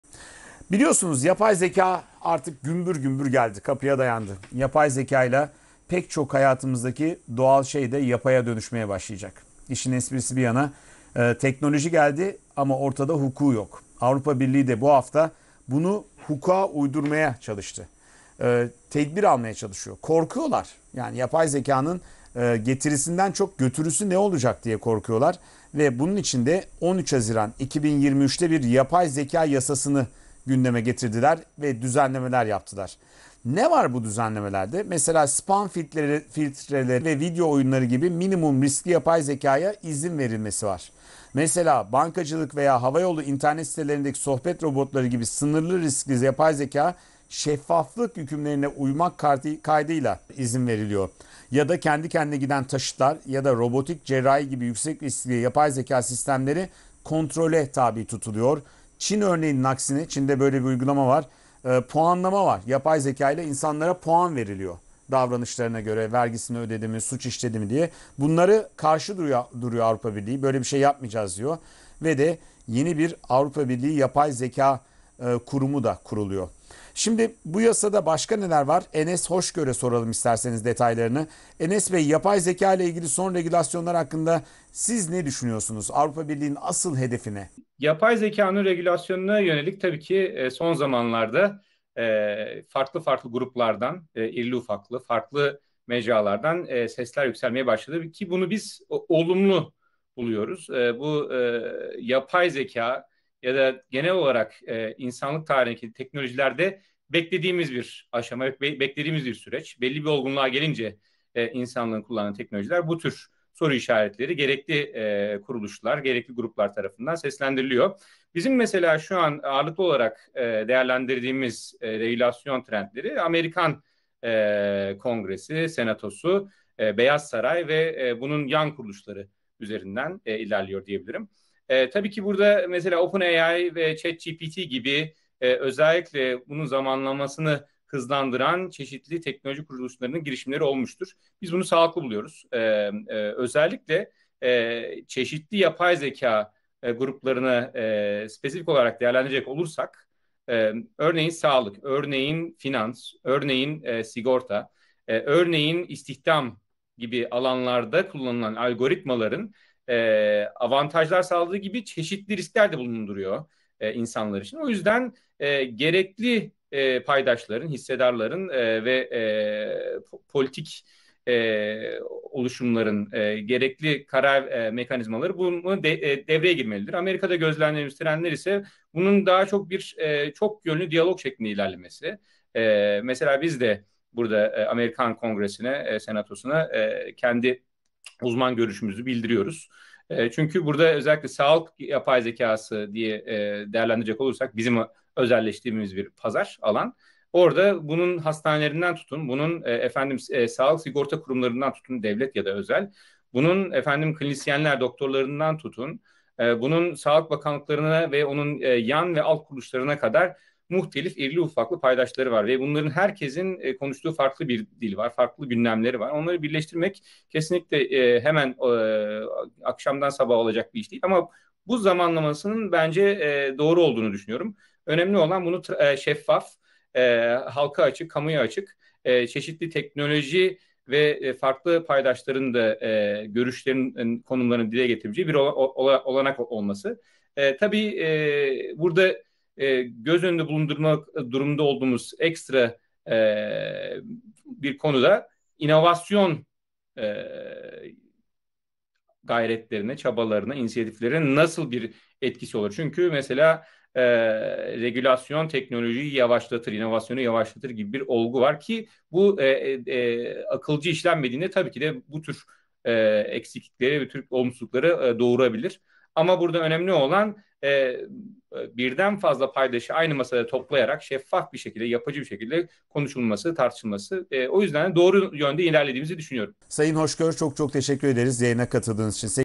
Avrupa Birliği yapay zeka yasası onaylandı! Enes Hoşgör'den dikkat çeken açıklamalar
Titan denizaltısının enkazı bulundu... Yunanistan'da göçmen teknesi faciası... Göçmenler Avrupa'ya nasıl ulaşıyor? ABD'de Hunter Biden krizi... Biden'ın oğlu suçlamaları kabul etti... Biden'ın rakibi Robert Kennedy Jr. kimdir? Avrupa Birliği yapay zeka yasası onaylandı... Teknolojinin kalbi VivaTech etkinliğinde atıyor... Cüneyt Özdemir 5N1K'da sordu; İGAM Başkanı Metin Çorabatır, Araştırmacı Yunus Emre Erdölen, Teknoloji Uzmanı Enes Hoşgör ve Melek Yatırımcı Semiramis Kulak yanıtladı.